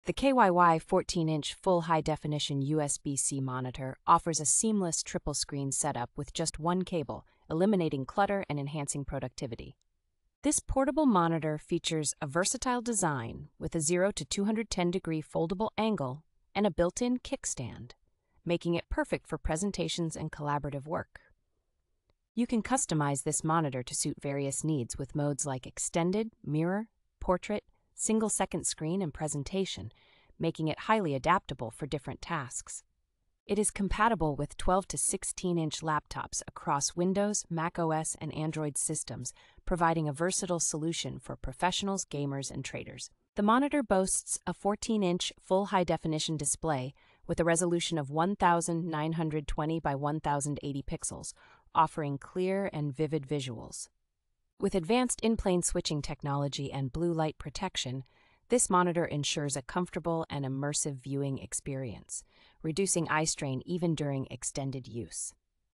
REVIEW (2025): KYY 14'' FHD USB-C Monitor. ESSENTIAL details.
The KYY 14 inch Full High Definition USB-C Monitor offers a seamless triple-screen setup with just one cable, eliminating clutter and enhancing productivity.

This portable monitor features a versatile design with a zero to two hundred ten degree foldable angle and a built-in kickstand, making it perfect for presentations and collaborative work.

You can customize this monitor to suit various needs with modes like extended, mirror, portrait, single second screen, and presentation, making it highly adaptable for different tasks.

It is compatible with twelve to sixteen inch laptops across Windows, macOS, and Android systems, providing a versatile solution for professionals, gamers, and traders.

The monitor boasts a fourteen inch Full High Definition display with a resolution of one thousand nine hundred twenty by one thousand eighty pixels, offering clear and vivid visuals.

With advanced In-Plane Switching technology and blue light protection, this monitor ensures a comfortable and immersive viewing experience, reducing eye strain even during extended use.